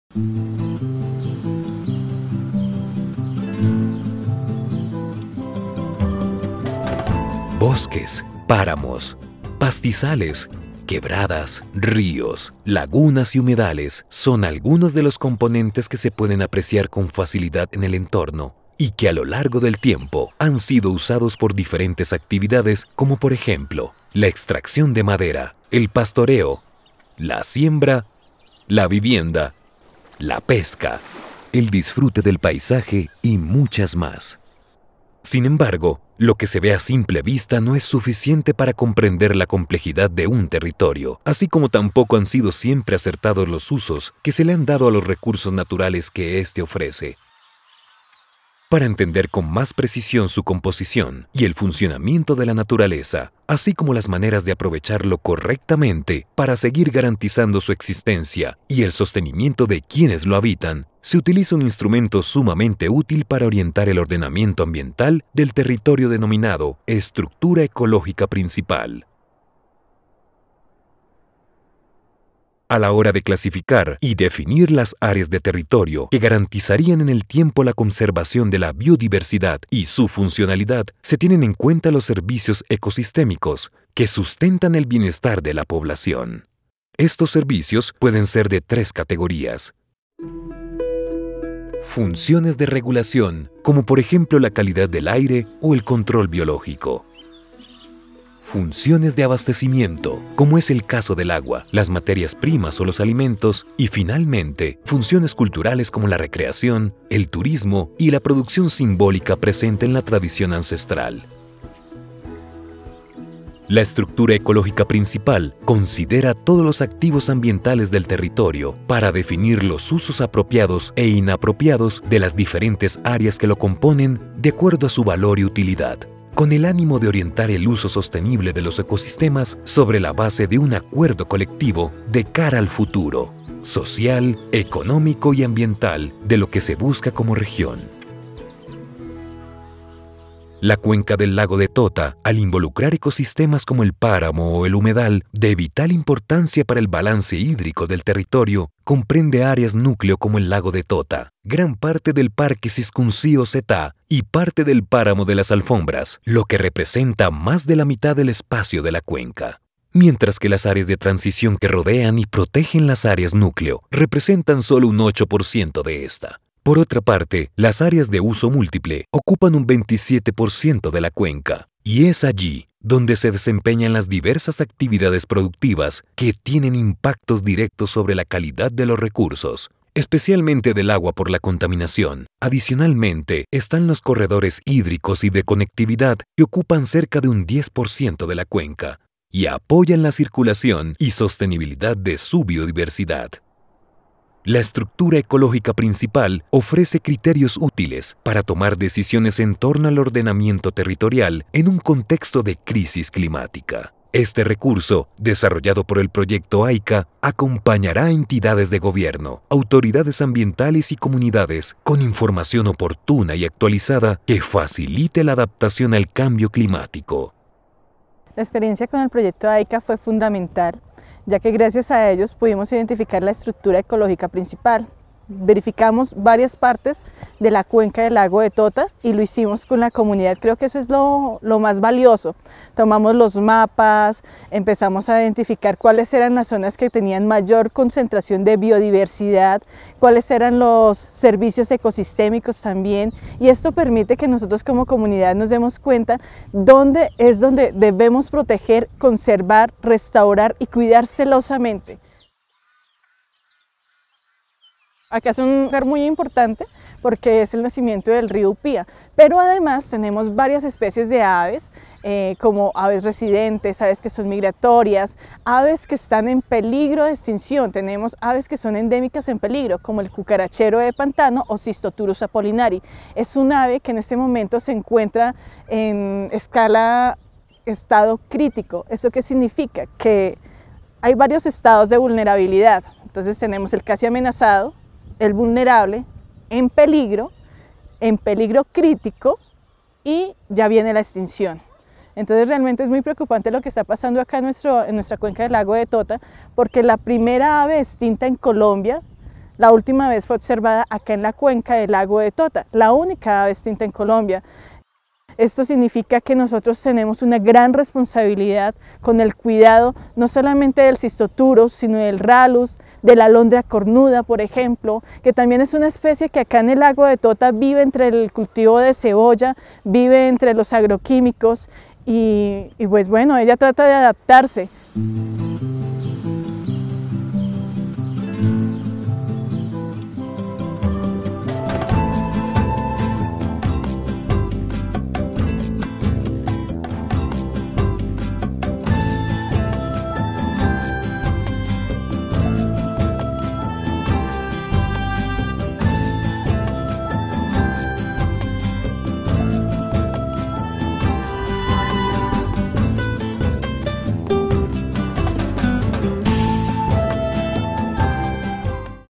Mi territorio, mi patrimonio - AICCA Colombia
Este material tiene por objetivo aportar en el entendimiento de y la importancia del cambio del clima, y como pueden ser las afectaciones en el territorio y las posibles consecuencias en las actividades productivas en la cuenca del Lago de Tota.